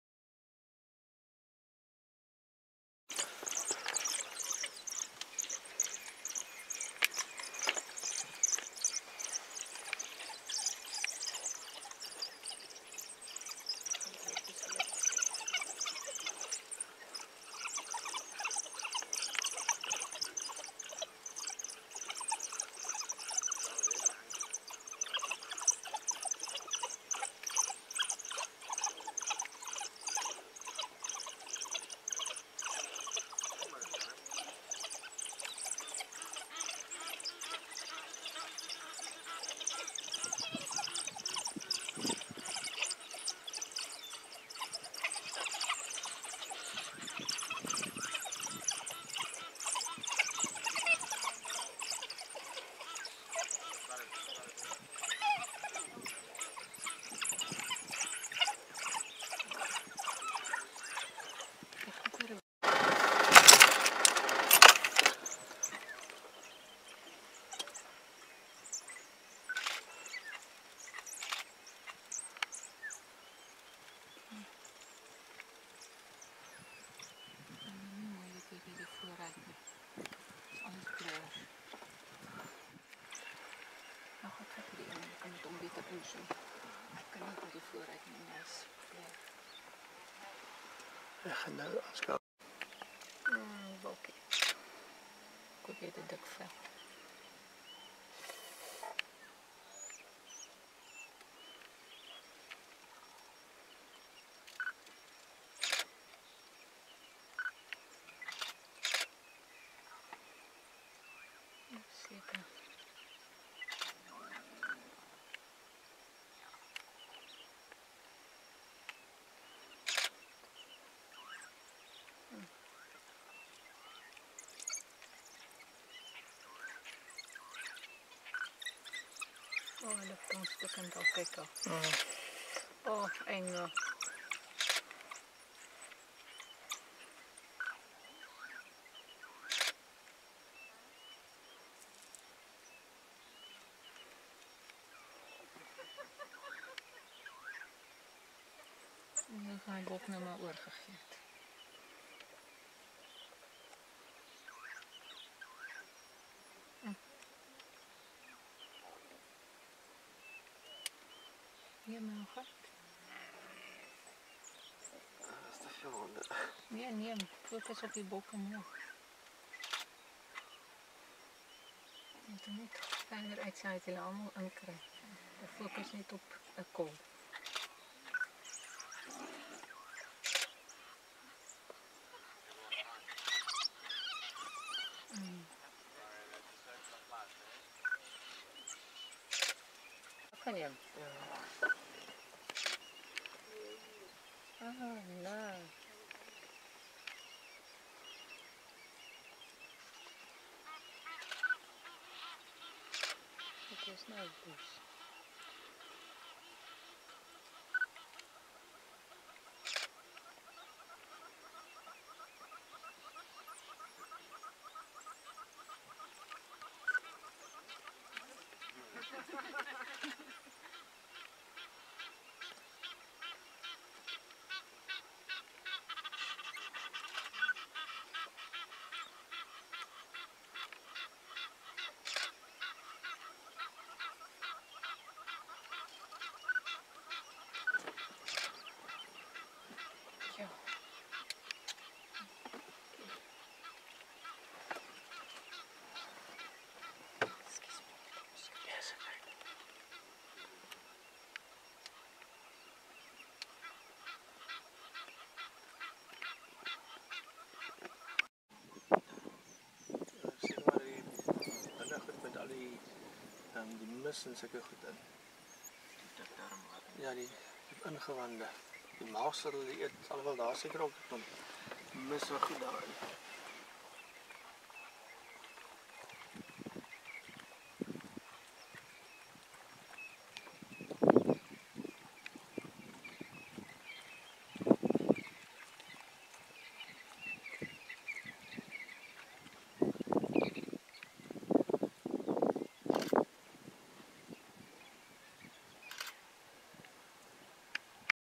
Pack of wild dogs taking waterbuck, toned down for sensitive viewers (full version on my channel)
This is an edited version of a previous post of a successful wild dog hunt and it may still be difficult to watch for sensitive viewers. I have removed some of the material that clearly made  some viewers unhappy. It still shows the realities of the cycle of life in a functional ecosystem where there is a food chain where some animals have to kill and others have to be killed on order to keep the pyramid in place and that we do not have plagues such as rat infestations.